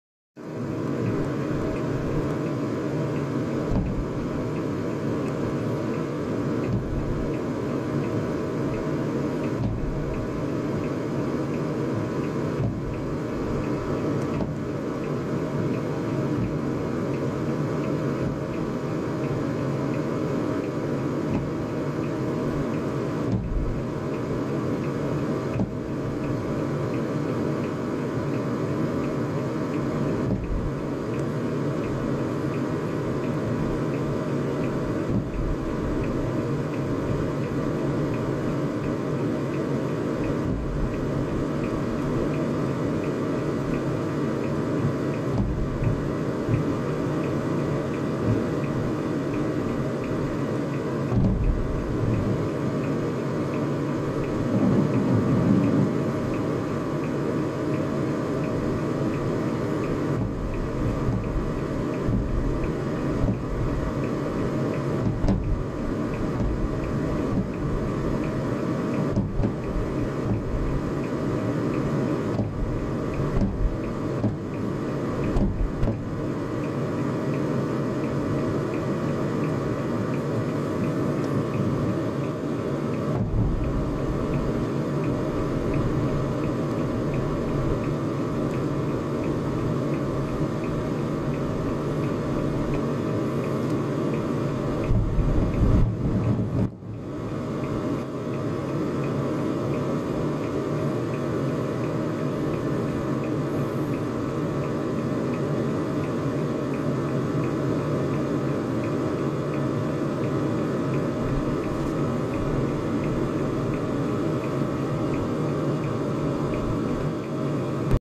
2023.8.15 木更津港まつり 花火大会2023
渋滞にハマった車内から撮影